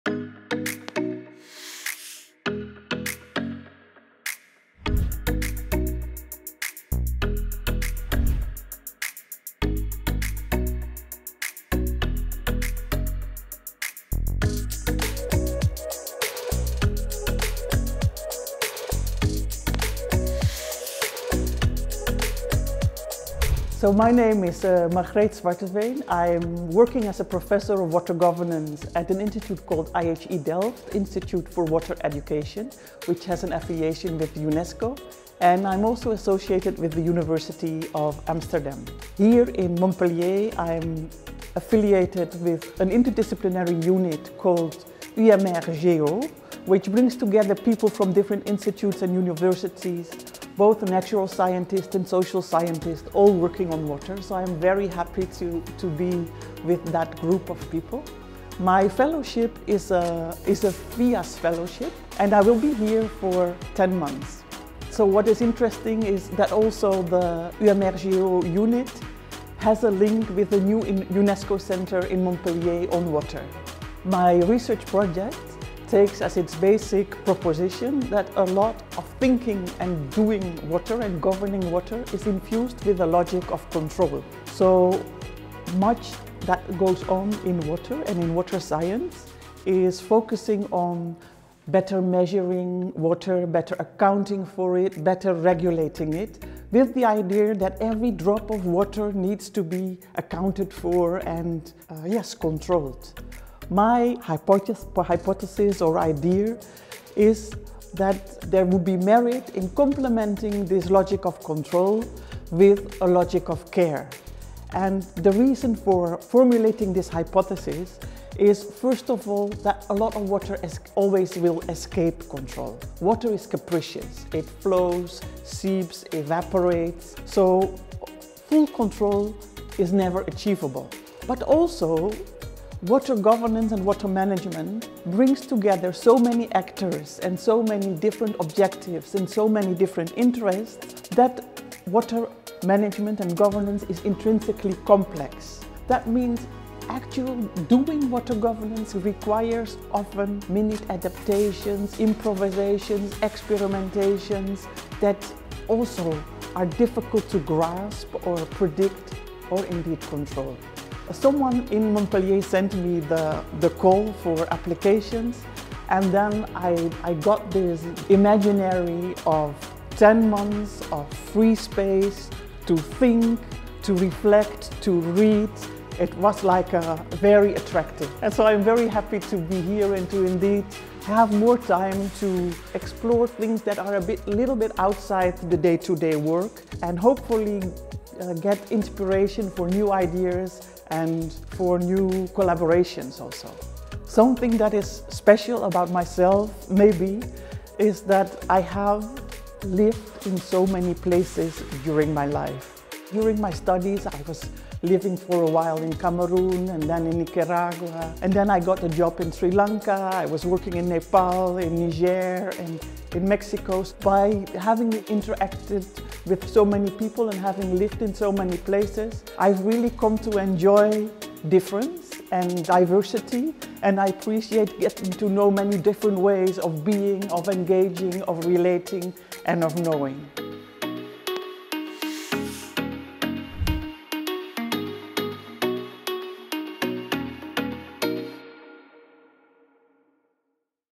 PORTRAIT / MAK'IT accueille MARGREET ZWARTEVEEN
Pour sa deuxième année d'activités, Montpellier Advanced Knowledge Institute on Transitions (MAK'IT) accueille, dans le cadre de ses dispositifs "SCIENTIFIQUES INVITES" et "FIAS", 8 nouveaux chercheurs internationaux.

𝗠𝗮𝗿𝗴𝗿𝗲𝗲𝘁 𝗭𝘄𝗮𝗿𝘁𝗲𝘃𝗲𝗲𝗻
Professeure en gouvernance de l’eau
Institut UNESCO-IHE de Delft pour l’éducation relative à l’eau (Pays-Bas)
𝘗𝘳𝘰𝘫𝘦𝘵 :  𝘚𝘦 𝘴𝘦𝘯𝘵𝘪𝘳 𝘤𝘰𝘯𝘤𝘦𝘳𝘯é 𝘱𝘢𝘳 𝘭𝘦𝘴 𝘱𝘳𝘢𝘵𝘪𝘲𝘶𝘦𝘴 𝘥𝘦 𝘨𝘦𝘴𝘵𝘪𝘰𝘯 𝘥𝘦 𝘭’𝘦𝘢𝘶 𝘦𝘵 𝘱𝘳𝘢𝘵𝘪𝘲𝘶𝘦𝘳 𝘶𝘯𝘦 𝘨𝘦𝘴𝘵𝘪𝘰𝘯 𝘤𝘰𝘯𝘤𝘦𝘳𝘯é𝘦 𝘥𝘦 𝘭’𝘦𝘢𝘶 : 𝘮𝘰𝘣𝘪𝘭𝘪𝘴𝘦𝘳 𝘭𝘦𝘴 𝘳é𝘧𝘭𝘦𝘹𝘪𝘰𝘯𝘴 𝘪𝘴𝘴𝘶𝘦𝘴 𝘥𝘦𝘴 é𝘵𝘶𝘥𝘦𝘴 𝘦𝘯 𝘴𝘤𝘪𝘦𝘯𝘤𝘦 𝘦𝘵 𝘵𝘦𝘤𝘩𝘯𝘰𝘭𝘰𝘨𝘪𝘦 𝘦𝘵 𝘥𝘦𝘴 é𝘵𝘶𝘥𝘦𝘴 𝘥é𝘤𝘰𝘭𝘰𝘯𝘪𝘢𝘭𝘦𝘴 𝘱𝘰𝘶𝘳 𝘥𝘺𝘯𝘢𝘮𝘪𝘴𝘦𝘳 𝘭𝘦𝘴 𝘤𝘰𝘭𝘭𝘢𝘣𝘰𝘳𝘢𝘵𝘪𝘰𝘯𝘴 𝘵𝘳𝘢𝘯𝘴𝘥𝘪𝘴𝘤𝘪𝘱𝘭𝘪𝘯𝘢𝘪𝘳𝘦𝘴 𝘢𝘤𝘤𝘰𝘮𝘱𝘢𝘨𝘯𝘢𝘯𝘵 𝘭𝘦𝘴 𝘵𝘳𝘢𝘯𝘴𝘧𝘰𝘳𝘮𝘢𝘵𝘪𝘰𝘯𝘴 𝘷𝘦𝘳𝘴 𝘭𝘢 𝘥𝘶𝘳𝘢𝘣𝘪𝘭𝘪𝘵é
𝘋𝘢𝘵𝘦𝘴 𝘥𝘦 𝘴é𝘫𝘰𝘶𝘳 : 16/09/2021 – 15/07/2022